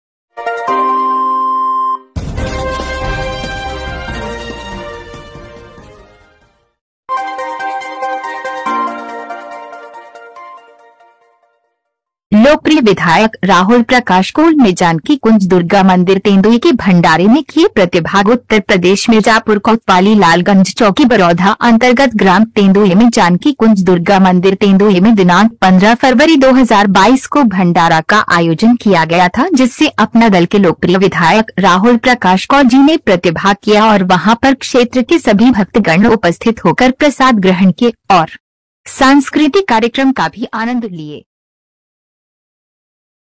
लोकप्रिय विधायक राहुल प्रकाश कोल ने जानकी कुन्ज दुर्गा मंदिर तेन्दुई के भण्डारे में किये प्रतिभाग
उत्तर प्रदेश मिर्जापुर कोतवाली लालगंज चौकी बरौधा अन्तर्गत ग्राम तेंदुई में जानकी कुन्ज दुर्गा मंदिर तेन्दुई में दिनांक 15फरवरी 2022को भण्डारा का आयोजन किया गया था जिससे अपना दल के लोकप्रिय विधायक राहुल प्रकाश कोल जी ने प्रतिभाग किया और वहां पर क्षेत्र के सभी भक्त गण उपस्थित होकर प्रसाद ग्रहण किए और सांस्कृतिक कार्यक्रम का भी आनंद लिए